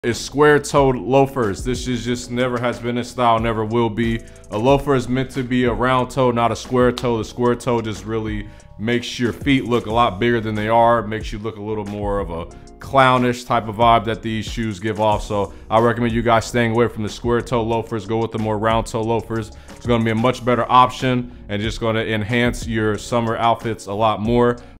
Don't Wear These Loafers styletip 2/10
In this short I will be going over loafers no man should wear if they want to improve their style. When you see these look the opposite direction!